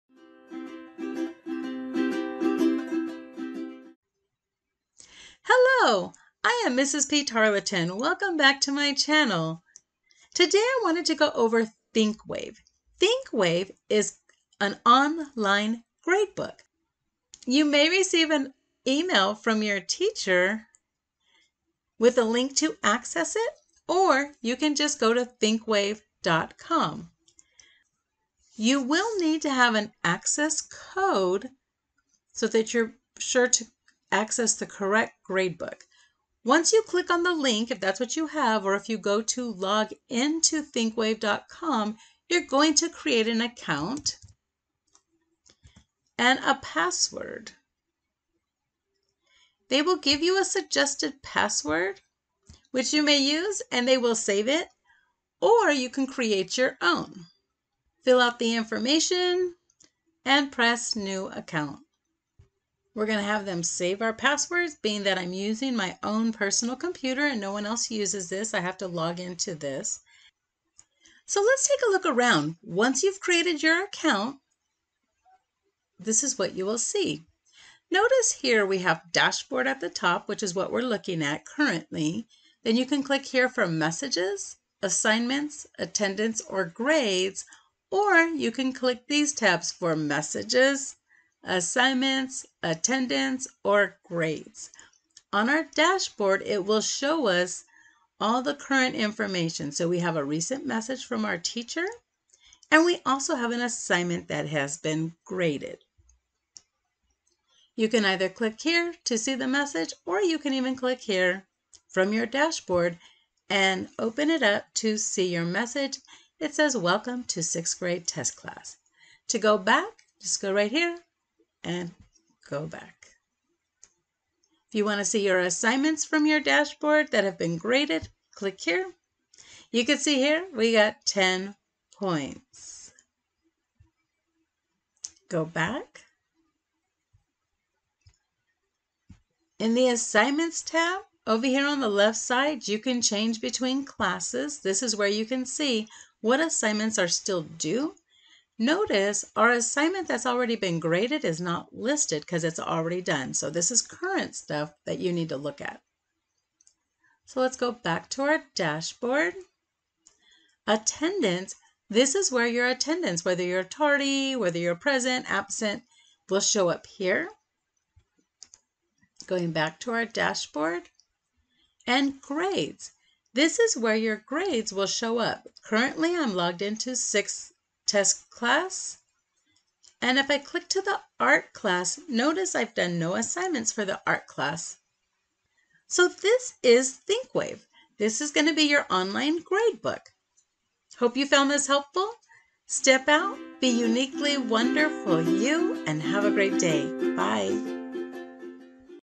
ThinkWave Online Gradebook
* In this tutorial I will give you an overview of ThinkWave Online Gradebook.

Take that step.  Be uniquely wonderful you.  You’ve got this.